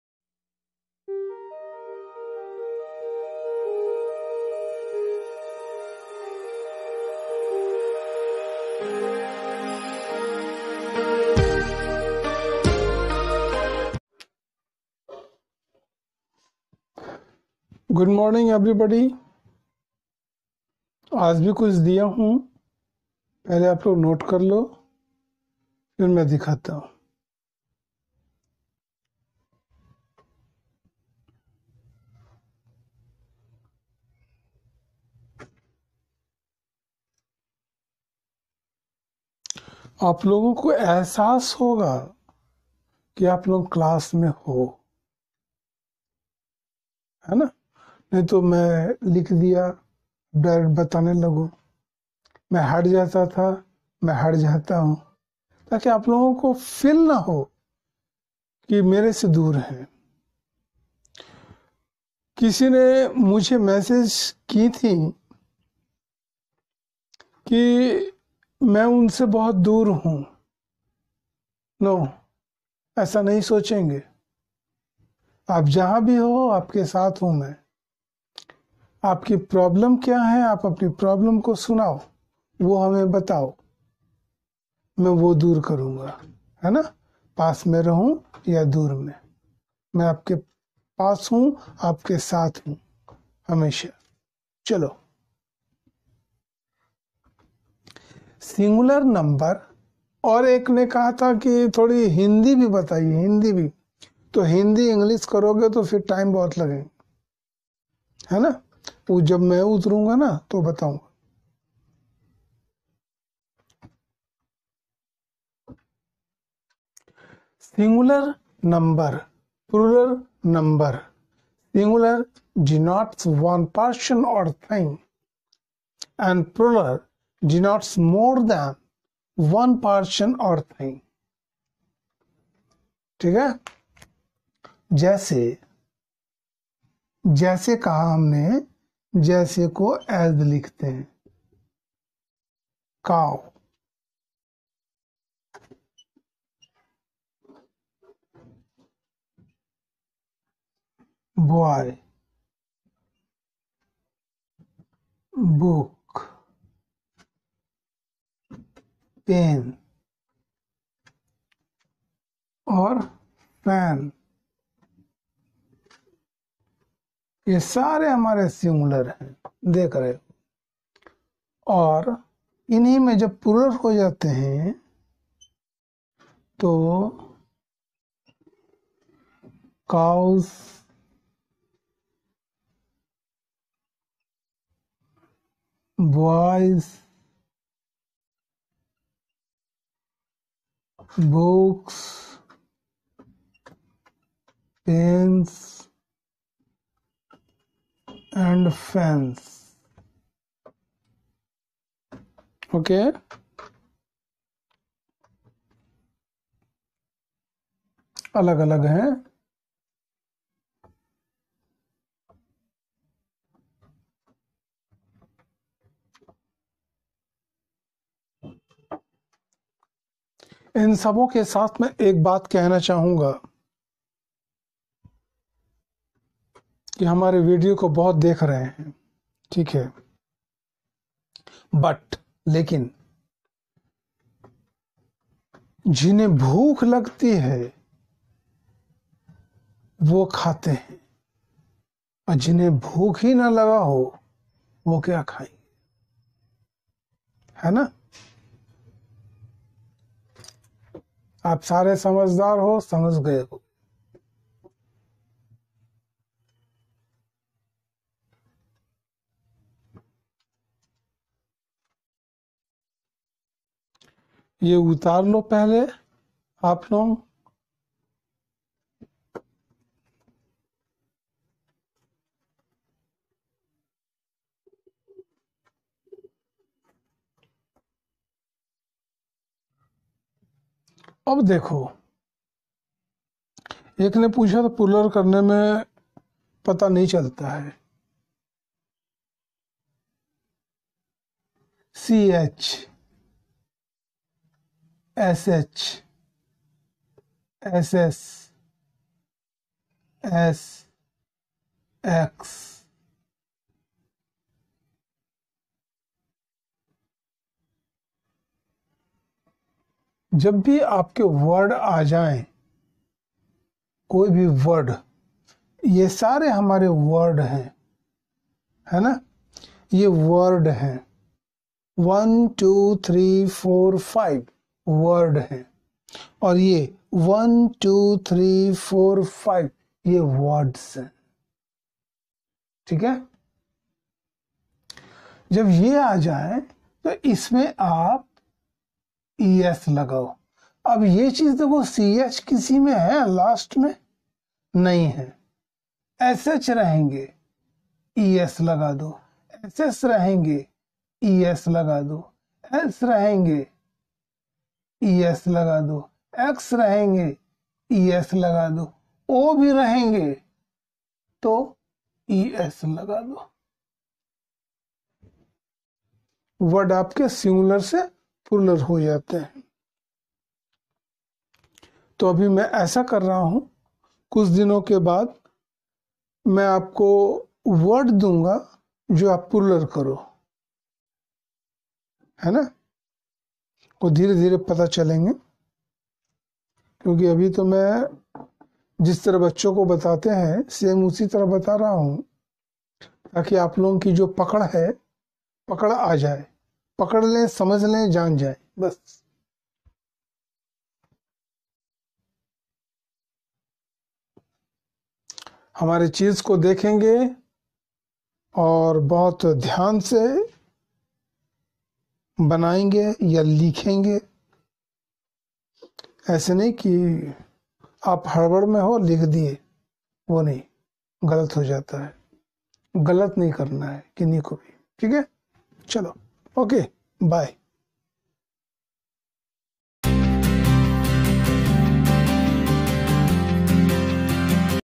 Concept of Singular number, Plural number
In this video of spoken English you will get to know about the concept of Singular number and plural number with some examples of it.